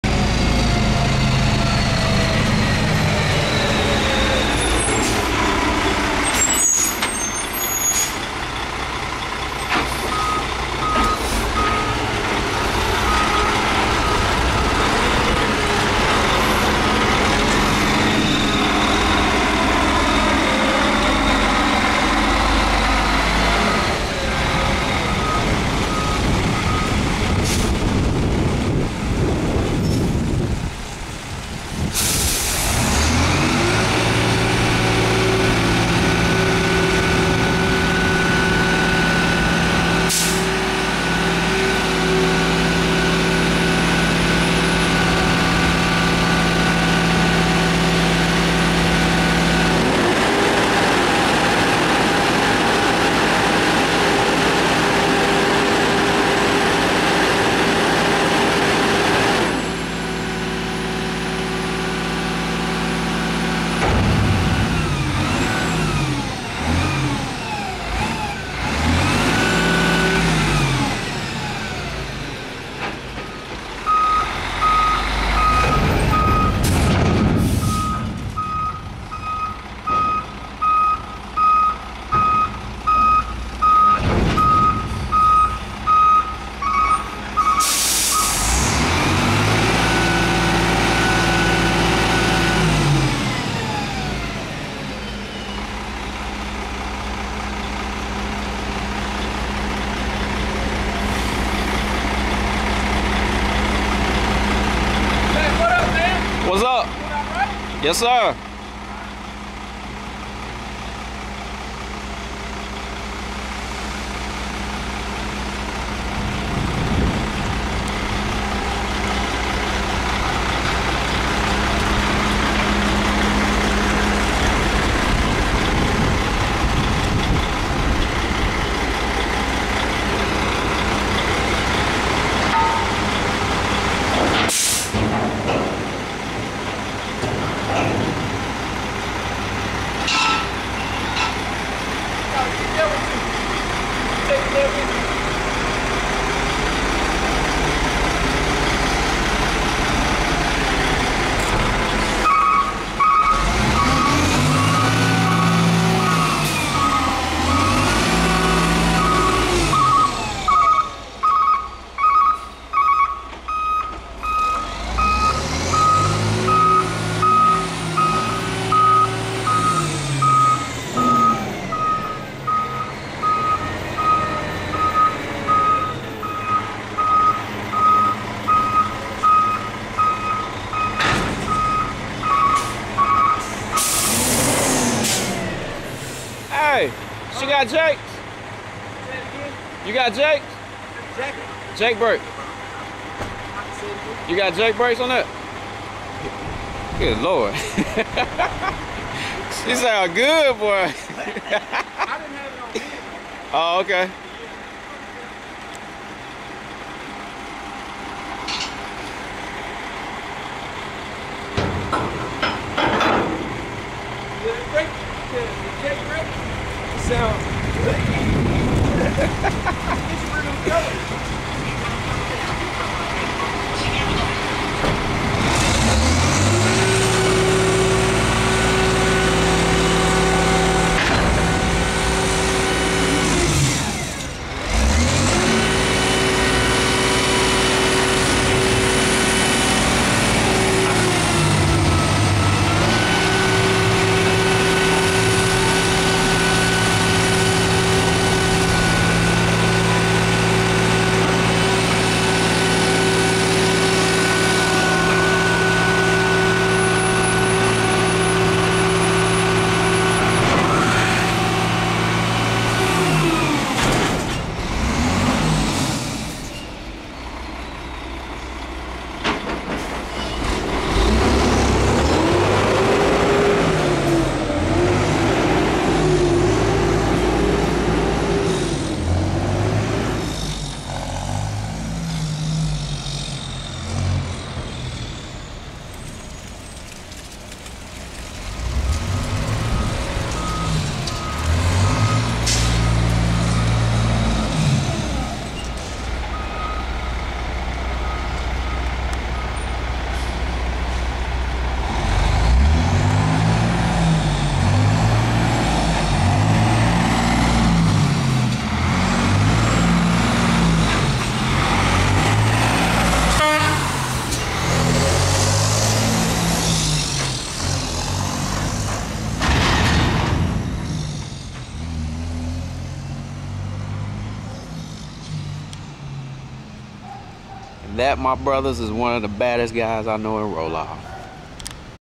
October 27, 2008 Starting Off Monday Right! Pt 1 Jermaine and His Freight Shaker
Managed to catch Jermaine again at Publix, returing their compactor and picking up an empty container...Love the engine on this truck!